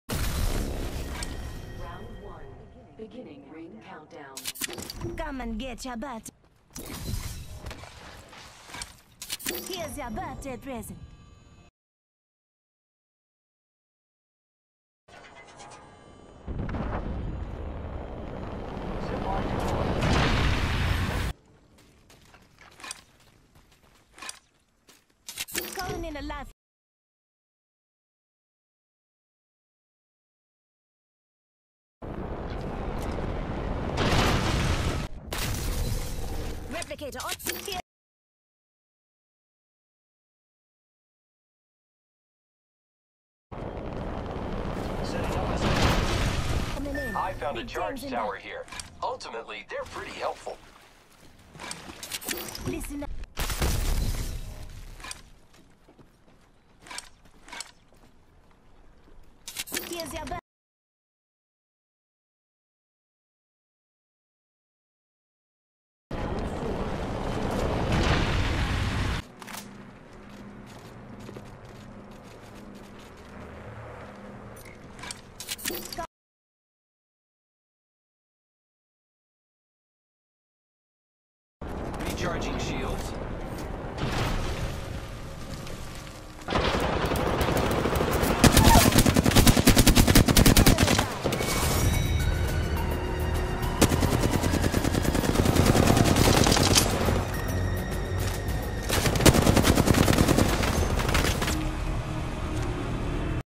How many care packages can I get in 1 game
I got 7 care packages if you weren't counting and were curious.